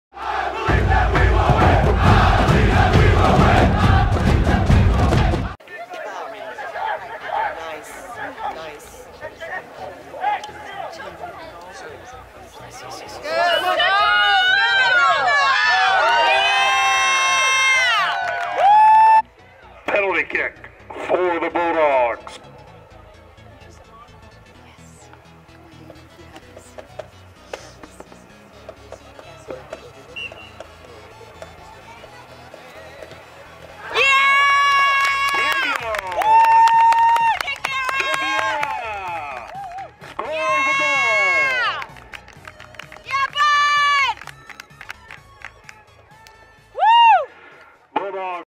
FCHS Varsity Soccer vs Pinecrest Feb 14 2017 Goals
FCHS Varsity Soccer (4)  vs Pinecrest (0)
Feb 14 2017
Goals